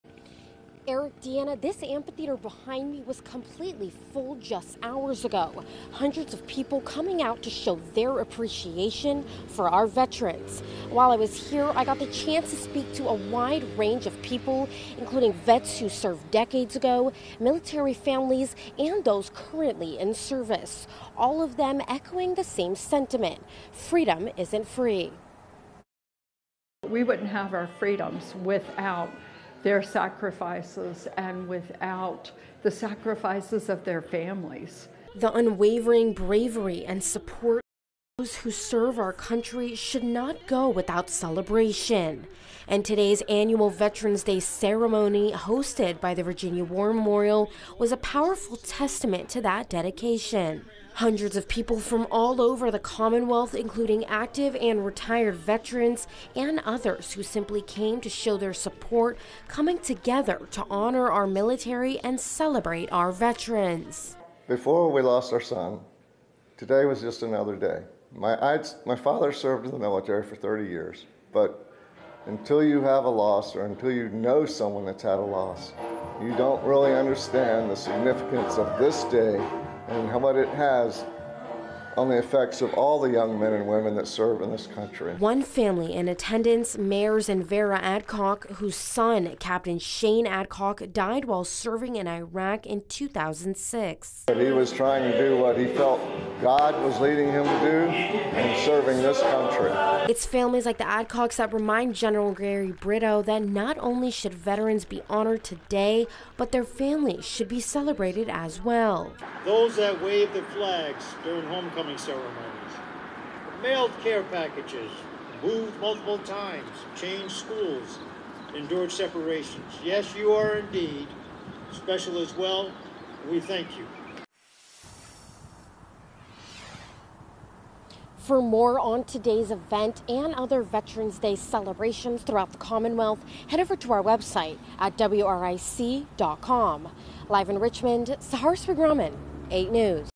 'We thank you': Veterans honored at Virginia War Memorial's 68th Annual Veterans Day Ceremony
Virginians across the Commonwealth gathered in Richmond on Monday, Nov. 11 to honor and reflect on the sacrifices of those who have served in the U.S. military.

The Virginia War Memorial hosted its 68th annual Veterans Day Ceremony on Monday, drawing hundreds of attendees to celebrate the courage and dedication of military men and women, both past and present.

The event, held in the Memorial’s outdoor amphitheater, was a moving tribute to those who have defended the freedoms Americans hold dear. The ceremony brought together veterans from all branches of the military, military families and active-duty service members, united in their recognition of the immense sacrifices made by those who serve.